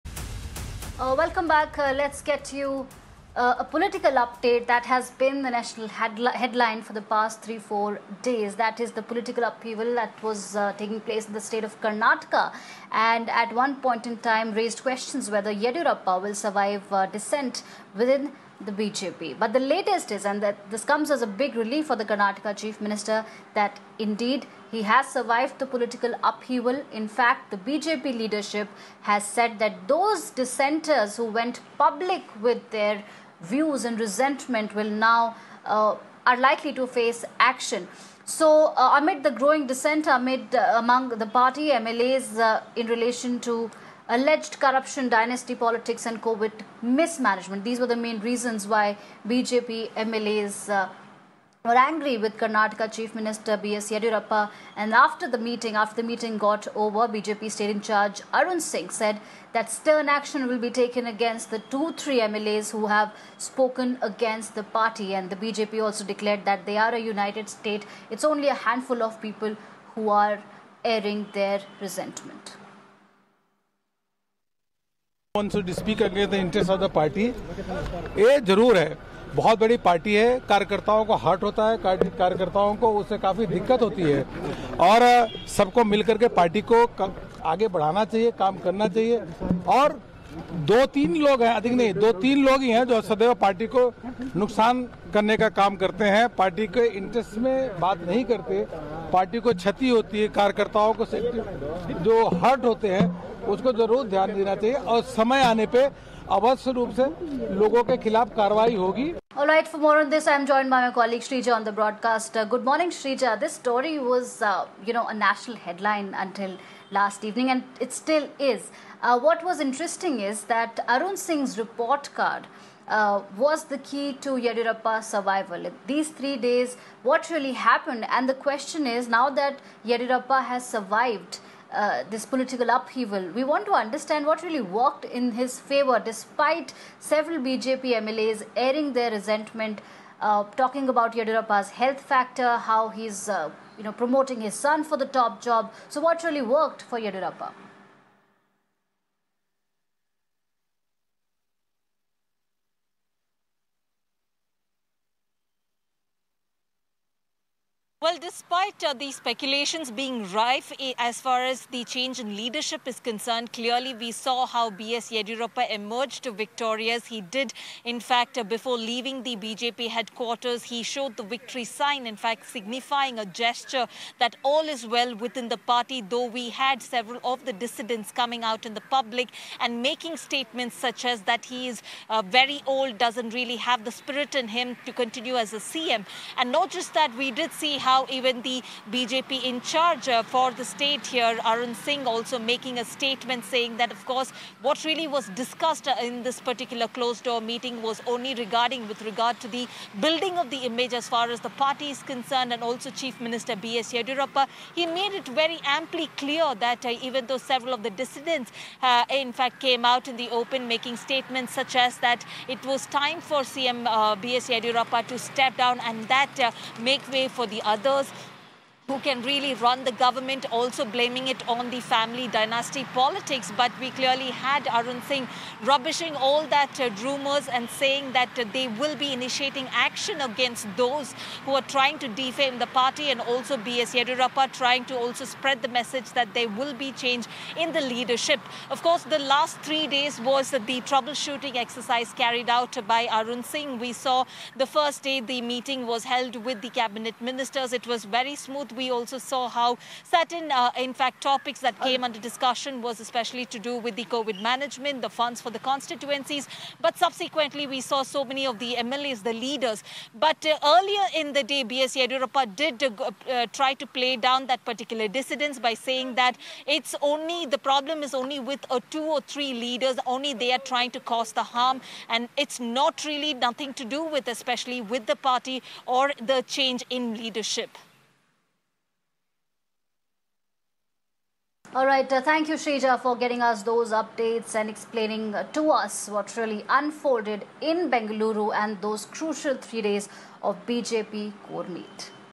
Karnataka Political crisis comes to an end; CM Yediyurappa to continue the leadership
The political upheaval in the state of Karnataka has finally come to rest now after Karnataka CM BS Yediyurappa faced constant criticism and whether he will survive the dissent or not. Karnataka CM BS Yediyurappa will continue to hold his power in the state after BJP's state in-charge Arun Singh assured that there are no talks of changing the party leadership in the state. The top brass of the BJP has gone ahead and stated that those dissenters who went public with their views and resentment, are likely to face action.

COVID mismanagement was one of the main reasons why the BJP MLAs in the state were angry with BS Yediyurappa and formed an anti-Yediyurappa faction.